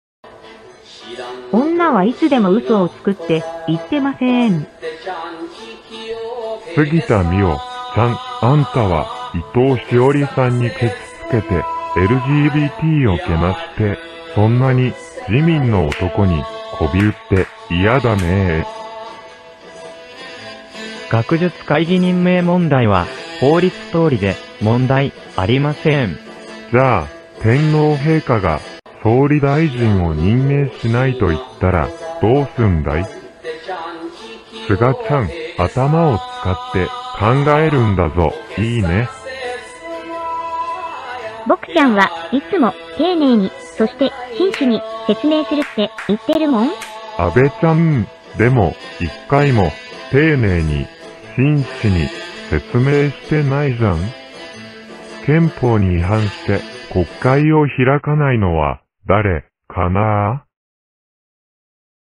杉田水脈＝”おんなはいつでも嘘つく” 自民安倍・菅の姑息・卑劣な、しらばっくれ論法だー
国会は何時になっても開かず、議論になってもはぐらかす、自民党の天に唾する伝統芸。